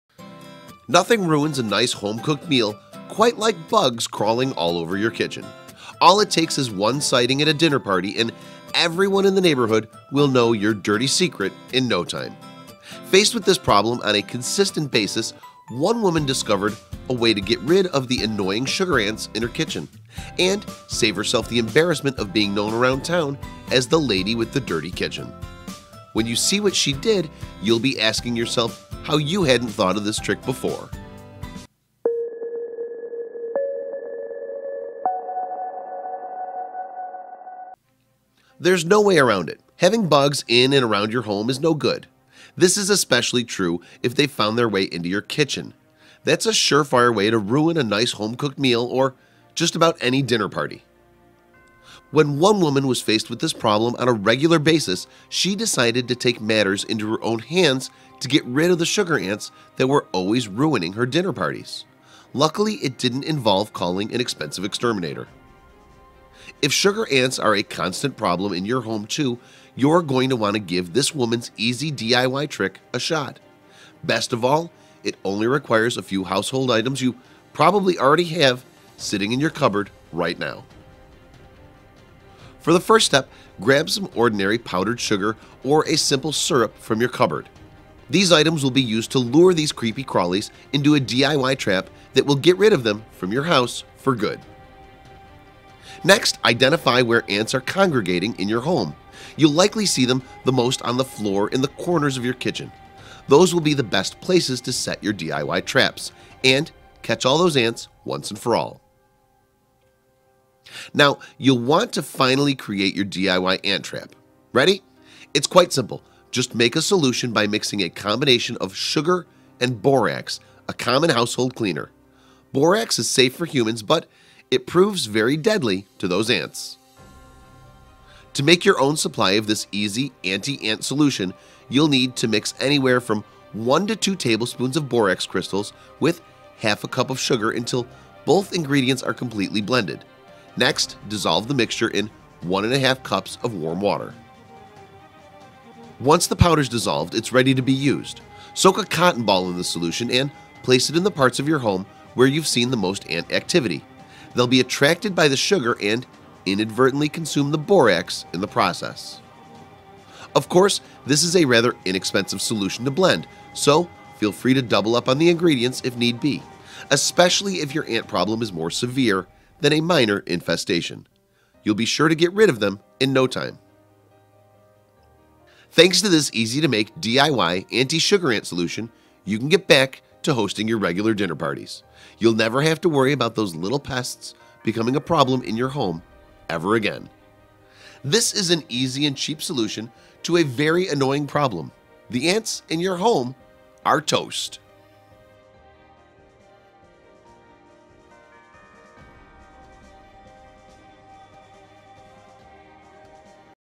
How to Get Rid of Ants Fast Naturally - DIY trick
Faced with this problem on a consistent basis, one woman discovered a way to get rid of the annoying sugar ants in her kitchen—and save herself the embarrassment of being known around town as the lady with the dirty kitchen.